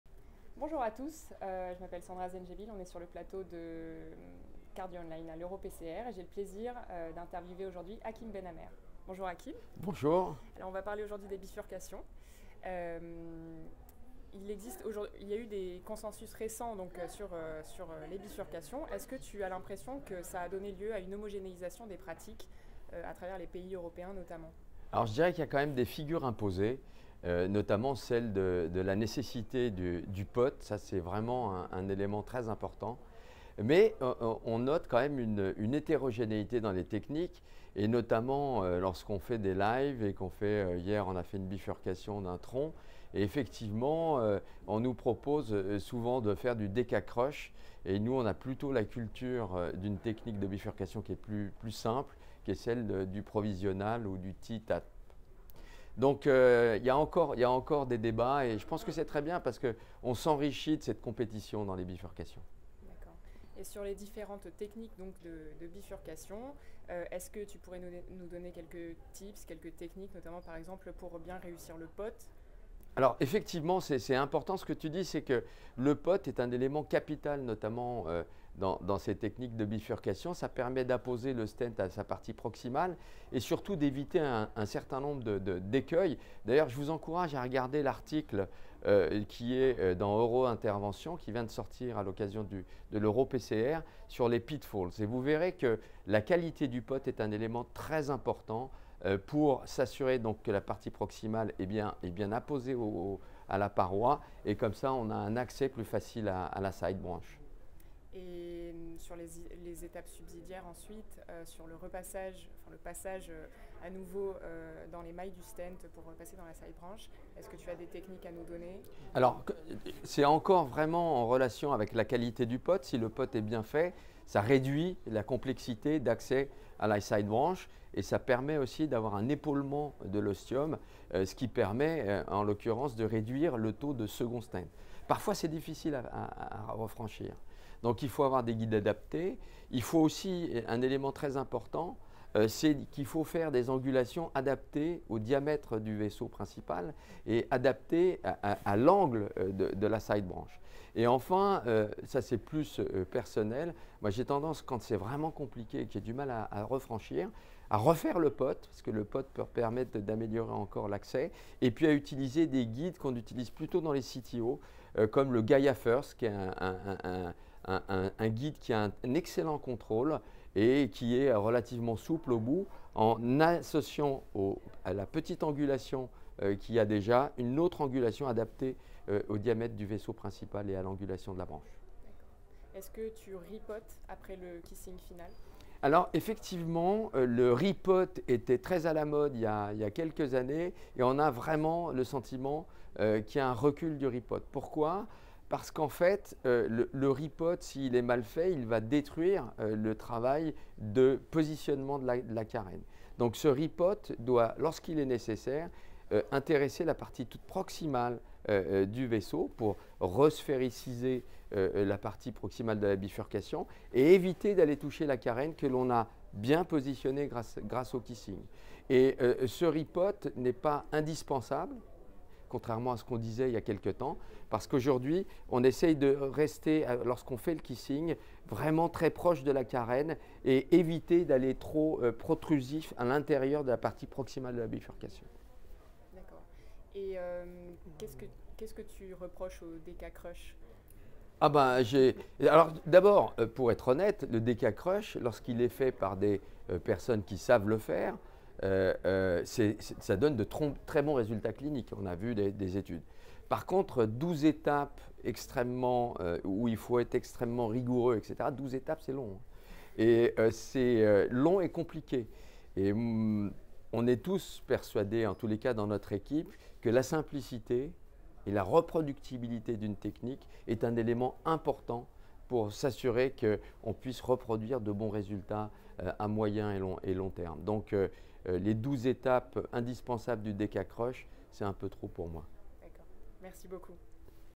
Consensus récents sur les bifurcations : vers une homogénéisation des pratiques en Europe ?
C'est lors d'EuroPCR 2022 qu'Hakim Benamer accorde un peu de son temps à Sandra Zendjebil pour parler bifurcations : si la nécessité du POT est indéniable, il explique qu'il existe toujours une hétérogénéité dans les techniques, qui permet débats et compétitions enrichissantes.
Il met ensuite l'accent sur la qualité du POT, élément capital, sur l'utilisation de guides adaptés, et sur la réalisation d'angulations adaptées au diamètre du vaisseau principal et à l'angle de la side-branch.
Enfin, il justifie le recul du re-POT par un meilleur Kissing, plus précis, et sa réticence envers le DK Crush et ses 12 étapes.